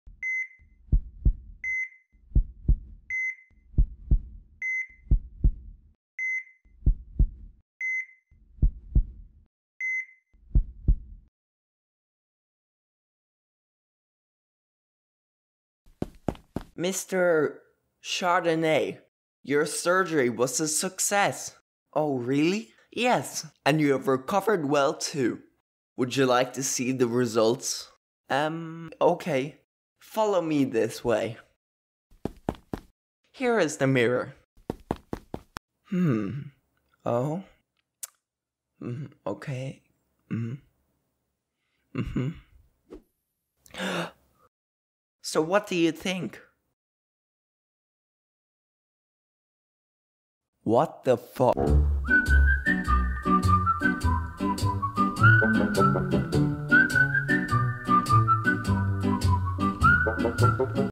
Grape Surgery 🍇
Mr. Chardonnay has undergone very rare Grape Surgery, where Surgeons did there best and " they did surgery on a grape "
Watch him witness the life changing improvements to his body.

Hastags